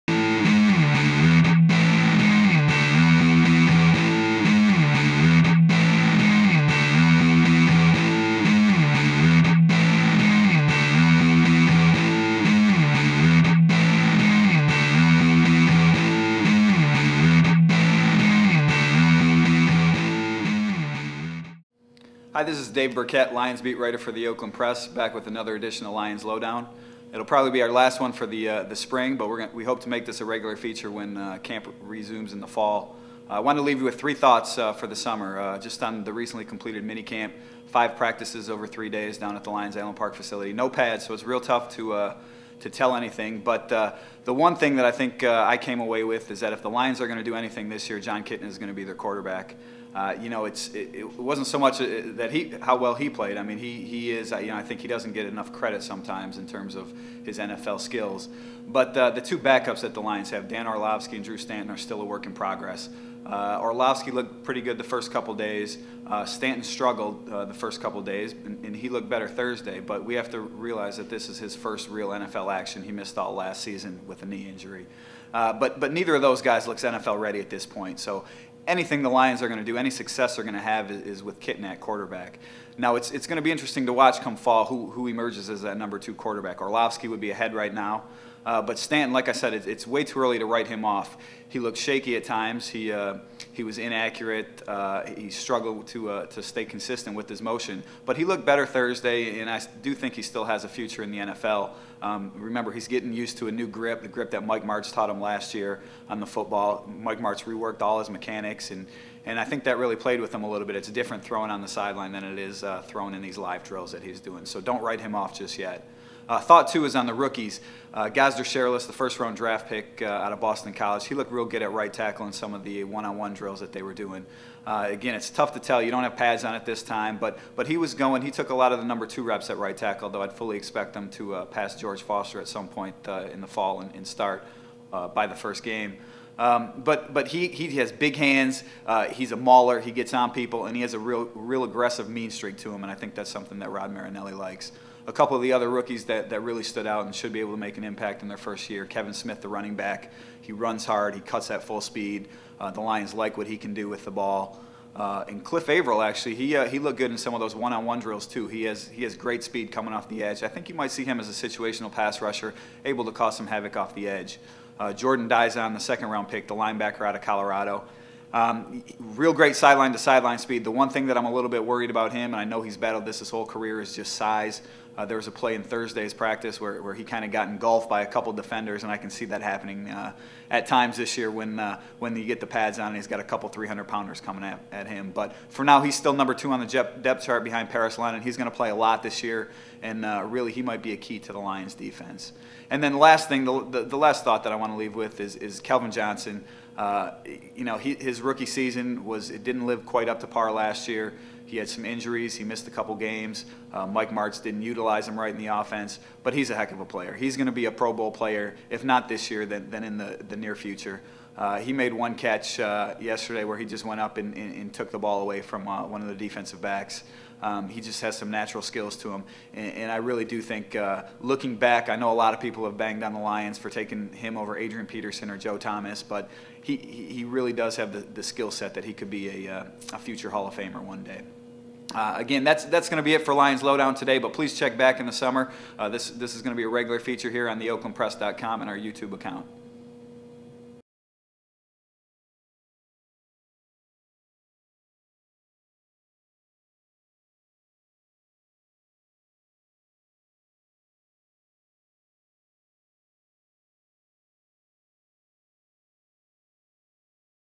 Lions Lowdown 523
Sports commentary on the Detroit Lions by The Oakland Press beat writer Dave Birkett. In this edition, Birkett discusses the Lions' QB issues, rookie performance and Calvin Johnson's second season.